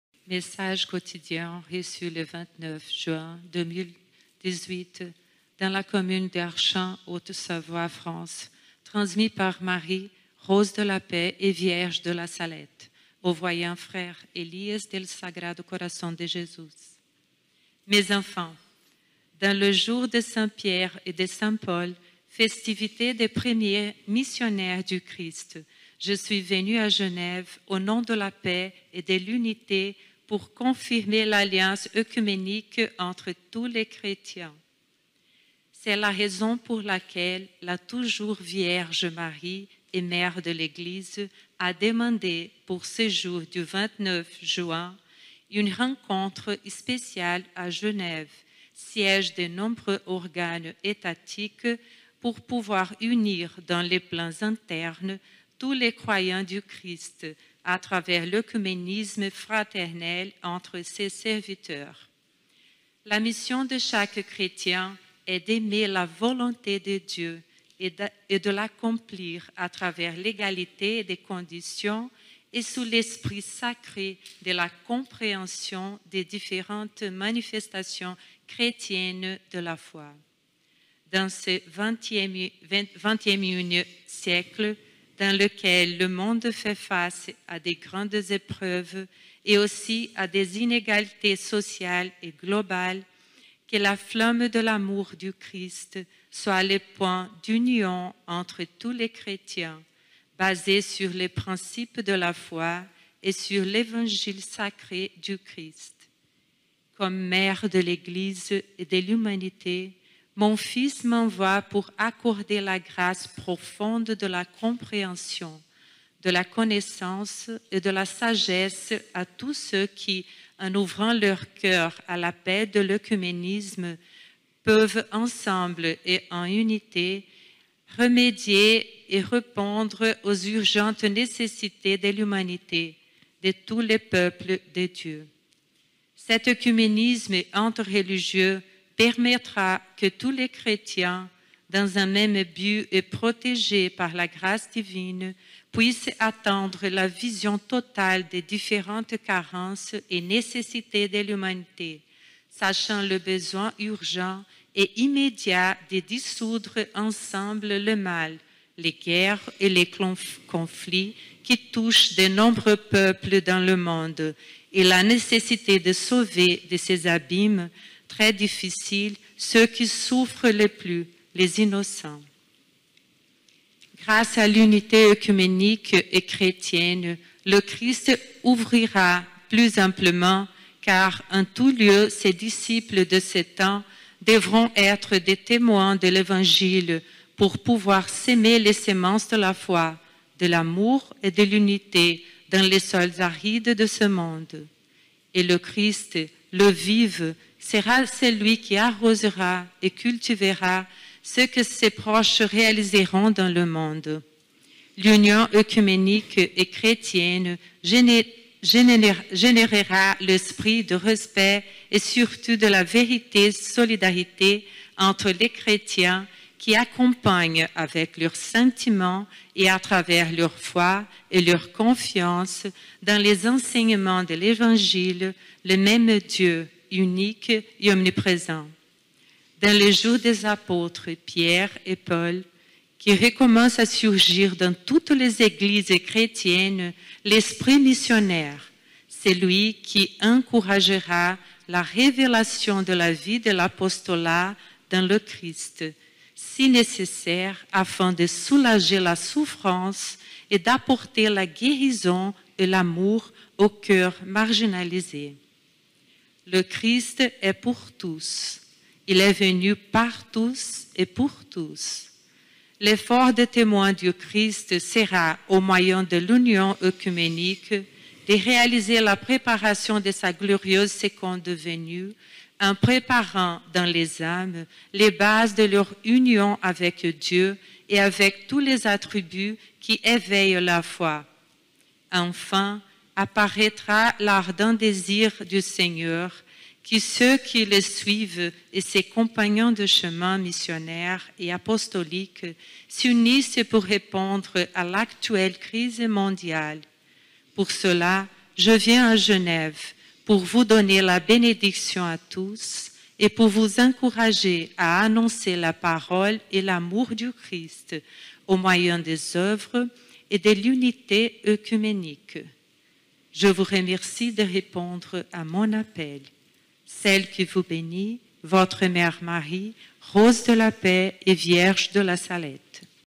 29/06/2018 - MESSAGE QUOTIDIEN DE MARIE / Daily Message of Mary
Message quotidien reçu le 29 juin 2018, dans la commune d'Archamps, Haute Savoie, France; transmis par Marie, Rose de la Paix et Vierge de La Salette, au voyant Frère Elías del Sagrado Corazón de Jésus.

Contenue dans/Contained in:

Apparition extraordinaire de la Vierge Marie / Extra Apparition of the Virgin Mary - 29/06/2018 - Genève, Suisse.

Video (unofficial) recorded from Misericordia Maria TV transmission by Wagner Silva